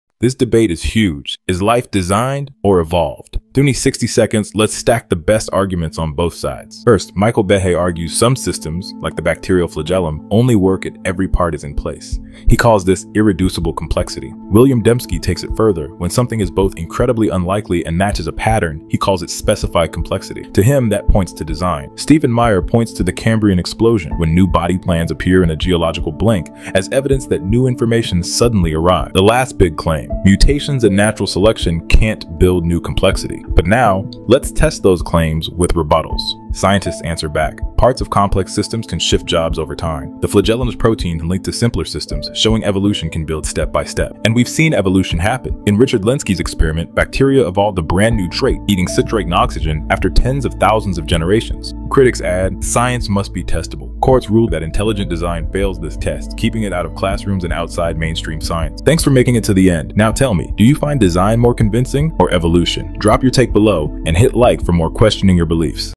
Evolution vs Intelligent Design
Is life designed… or evolved? In just 60 seconds, this video breaks down the top Intelligent Design arguments—like Michael Behe’s irreducible complexity, William Dembski’s specified complexity, and Stephen Meyer’s take on the Cambrian explosion. Then we look at the scientific rebuttals: how evolution can co-opt simpler systems, real-time evidence from Lenski’s long-term E. coli experiment, and why the Cambrian explosion unfolded over tens of millions of years, not in an instant.

We also touch on the limits of mutation and natural selection claim, and why critics argue Intelligent Design isn’t testable as science—a key reason it was ruled against in the Kitzmiller v. Dover trial.

This is a fast, clear breakdown of one of the biggest debates in science and philosophy. Whether you lean toward Intelligent Design or evolutionary biology, you’ll find the strongest points from both sides in one place.

👉 What do you think? Does complexity prove design—or can evolution explain it all?